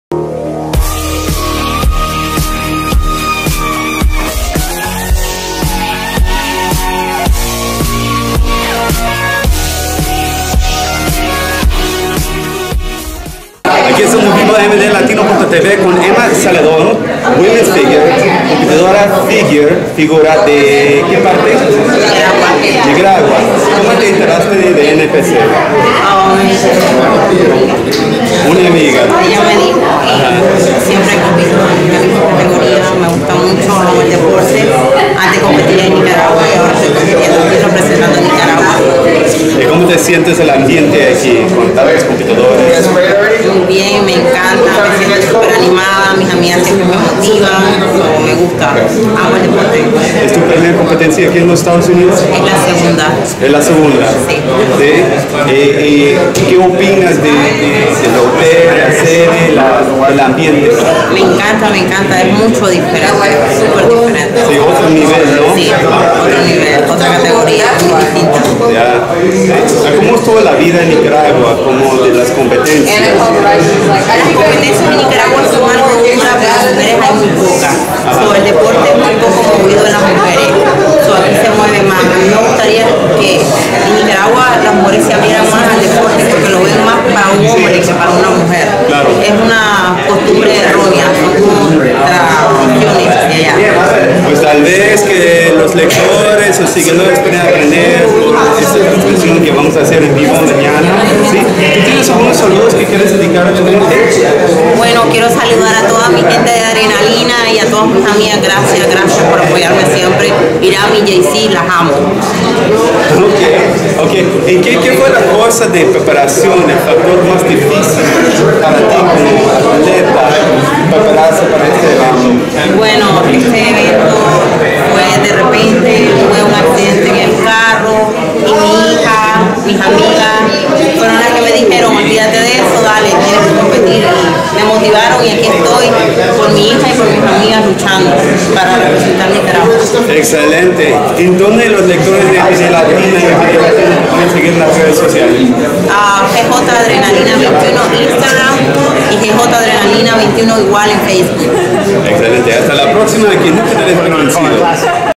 Gema Zeledon Miami Muscle Beach Pro 2015
Kit Sanderson de MD Latino presenta a Gema Zeledon antes del evento Miami Muscle Beach Classic 2015. carrito.mdlatino.com